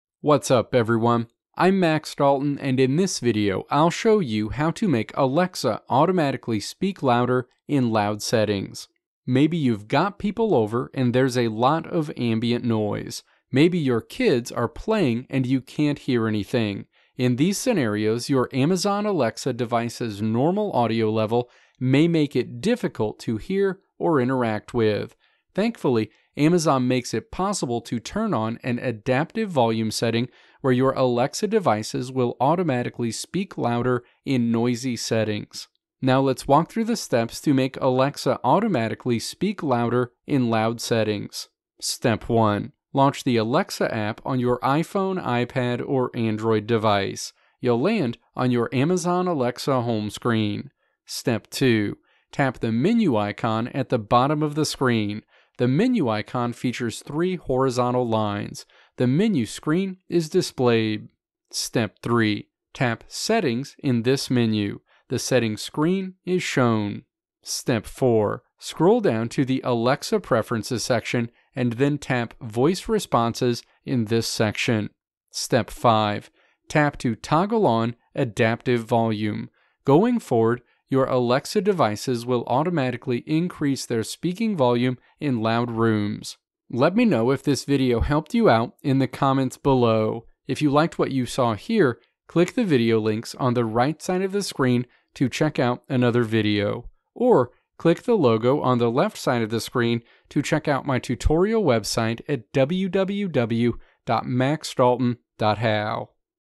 How to Make Alexa Automatically Speak Louder in Loud Settings
In this video I'll show you how to make Alexa automatically speak louder in loud settings.

Timestamps:
Introduction: 0:00
Steps to Change Font Size in Twitter: 0:35
Conclusion: 1:26

Video Transcript:
Maybe you've got people over and there's a lot of ambient noise. Maybe you're kids are playing and you can't hear anything. In these scenarios your Amazon Alexa device's normal audio level may make it difficult to hear or interact with. Thankfully, Amazon makes it possible to turn on an adaptive volume setting where your Alexa devices will automatically speak louder in noisy settings.

Now let's walk through the steps to make Alexa automatically speak louder in loud settings.

Step 1. Launch the Alexa app on your iPhone, iPad or Android device. You'll land on your Amazon Alexa home screen.

Step 2. Tap the "Menu" icon at the bottom of the screen. The Menu icon features three horizontal lines. The Menu screen is displayed.

Step 3. Tap "Settings" in this menu. The Settings screen is shown.

Step 4. Scroll down to the Alexa Preferences section, and then tap "Voice Responses" in this section.

Step 5. Tap to toggle on "Adaptive Volume." Going forward, your Alexa devices will automatically increase their speaking volume in loud rooms.